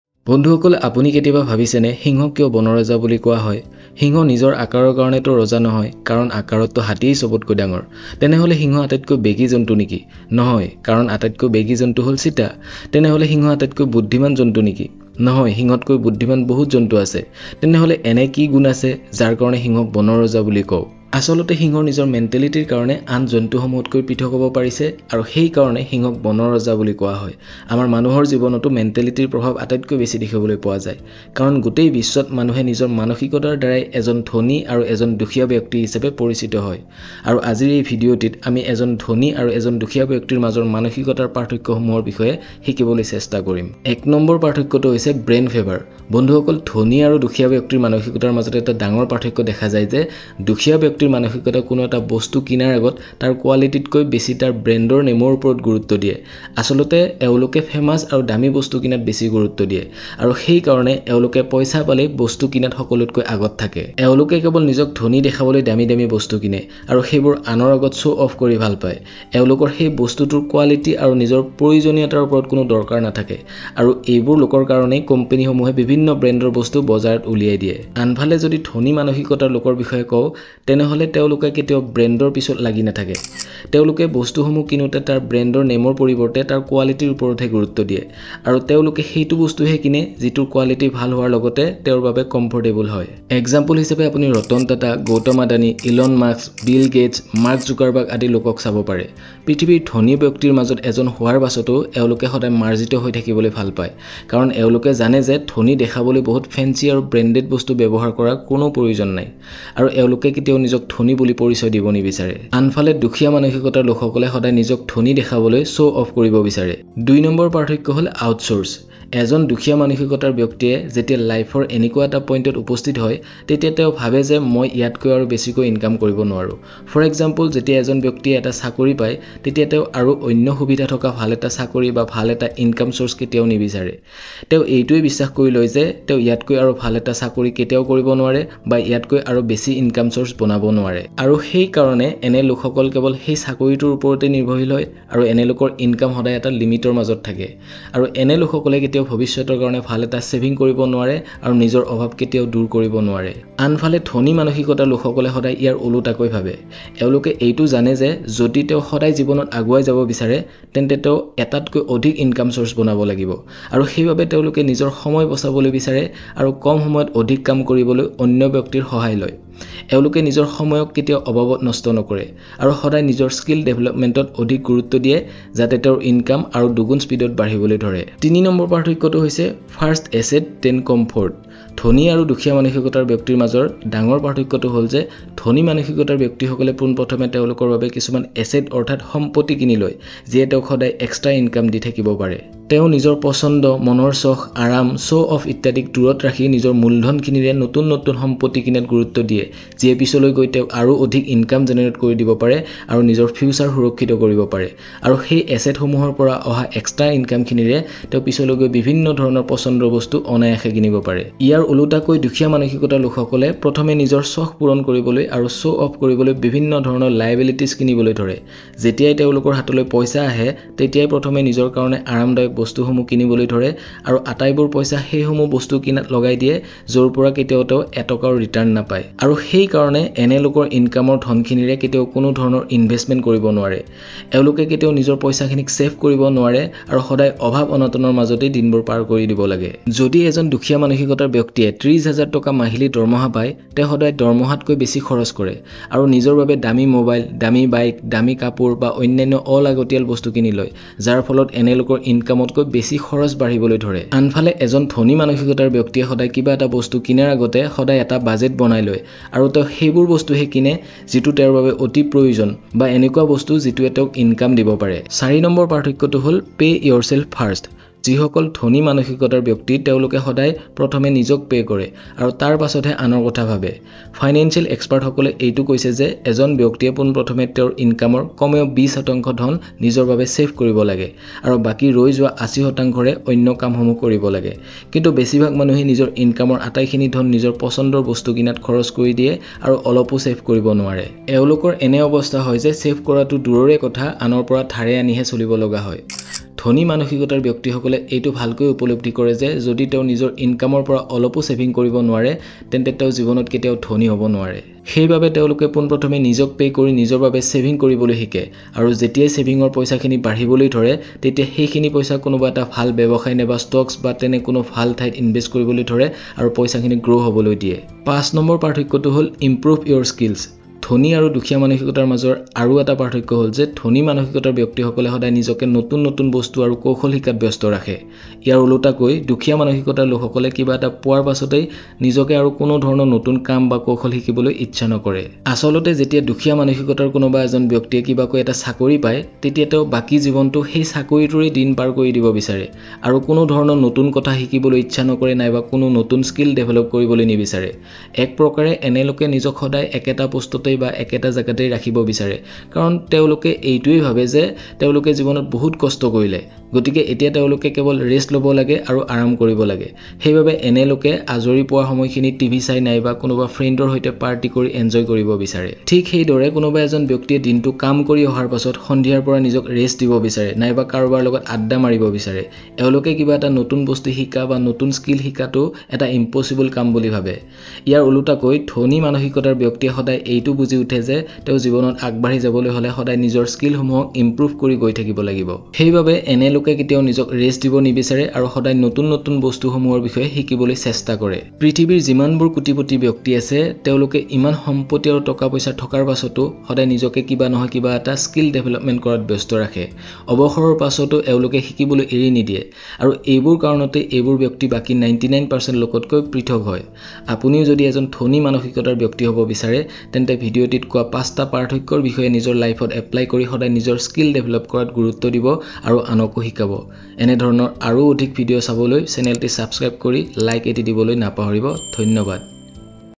ধনী আৰু দুখীয়াৰ মাজত ৫টা কি পাৰ্থক্য আছে|| 5 Differences between Rich and Poor Mindset
In this video we seen 5 differences between rich and poor mindset people which indicated our thoughts and habits of our personal financial achievement. First is brand fever, second is outsources, third one is about assets and liabilities, 4th is pay yourself first and last one improve skills.